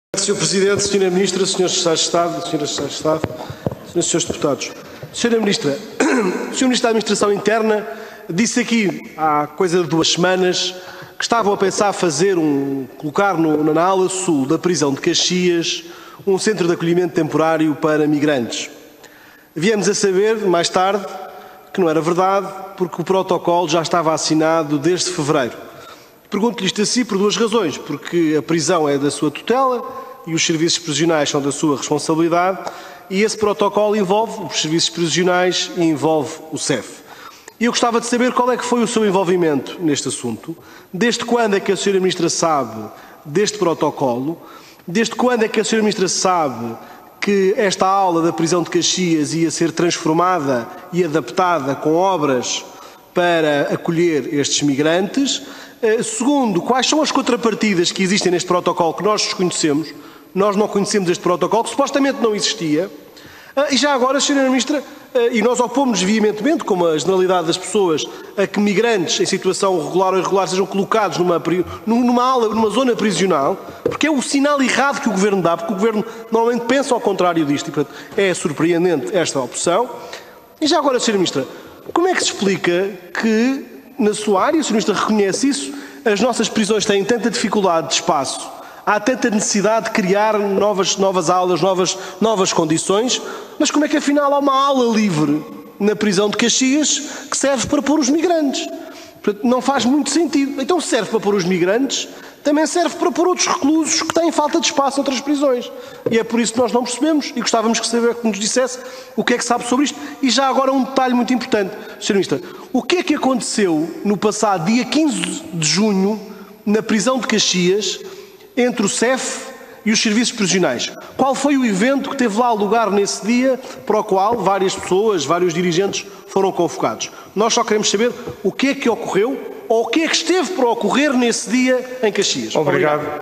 Duarte Marques questiona a Ministra da Justiça
Intervenção na Comissão de Assuntos Constitucionais, Direitos, Liberdades e Garantias.
23 junho 2021